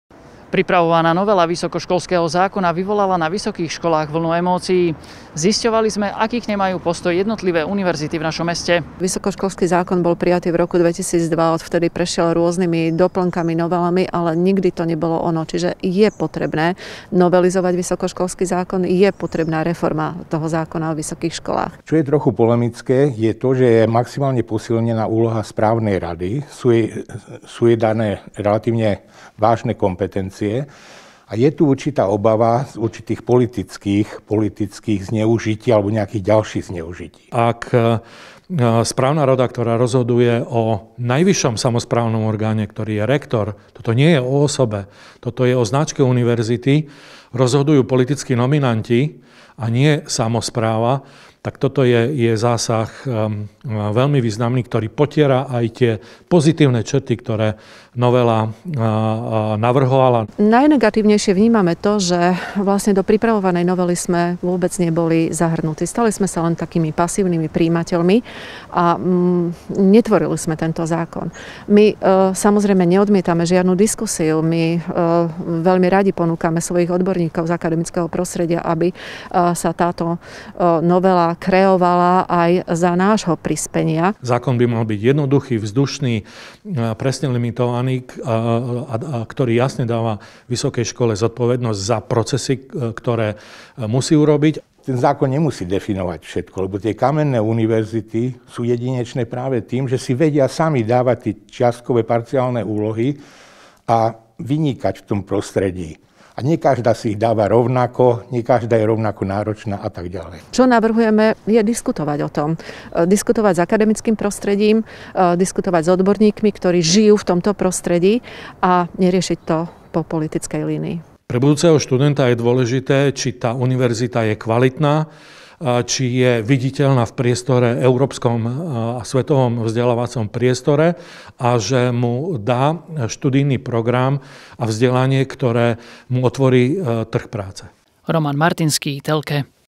Postoj košických univerzít k novele VŠ zákona
Novela zákona o vysokých školách má radikálne zasiahnuť do systému vysokoškolského vzdelávania. Voči tomu sa ohradzujú predstavitelia akadademickej obce a univerzity volajú po dialógu medzi Ministerstvom školstva a vysokými školami. Zisťovali sme postoj jednotlivých univerzít v našom meste.